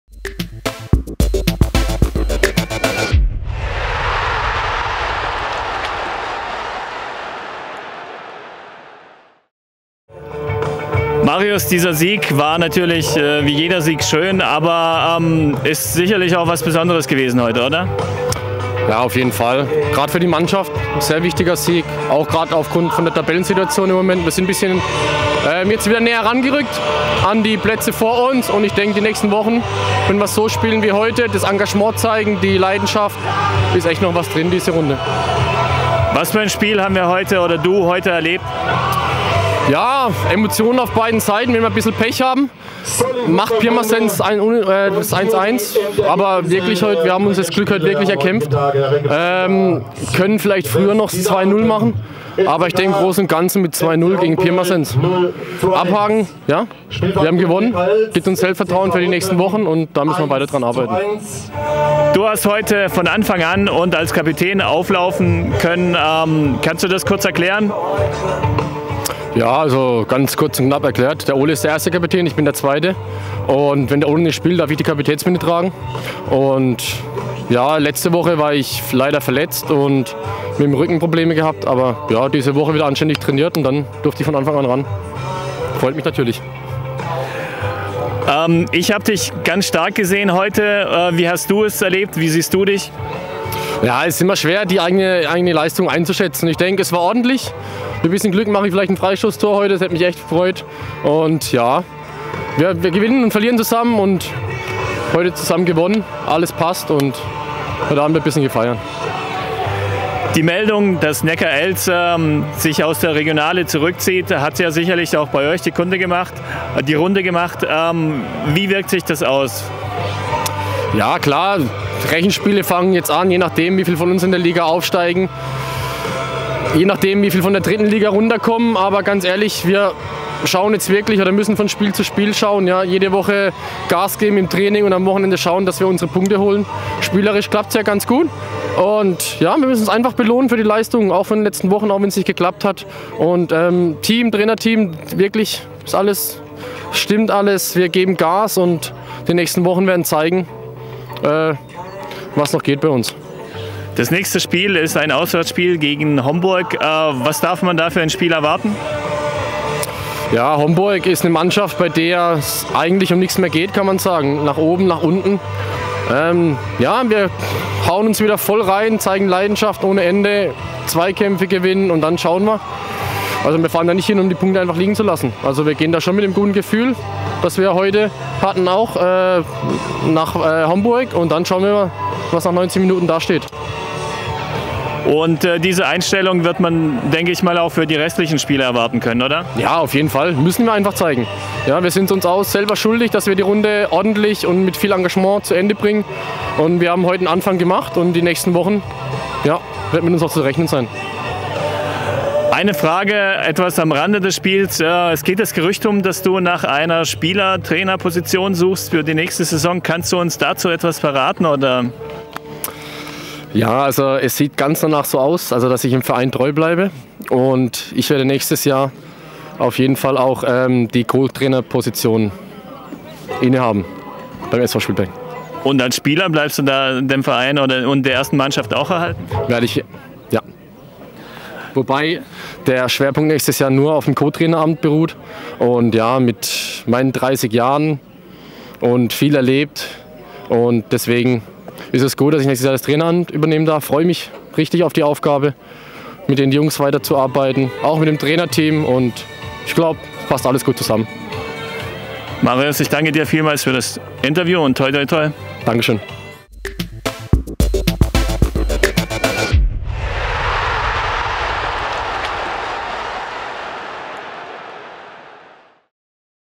Marius Mößner - SV Spielberg - zum Spiel vs. FK 03 Pirmasens, im FuPa.tv-Interview am 16.4.2016
Marius Mößner, Co-Kapitän des SV Spielberg, im FuPa.tv-Interview zum Spiel SV Spielberg vs. FK 03 Pirmasens am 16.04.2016, dem 28 Spieltag der Regionalliga Südwest 2015/16.

Der Mittelfeldspieler äußert sich zum wichtigen Sieg im Abstiegskampf und auch dazu, wie es für ihn persönlich nächste Saison weitergehen wird.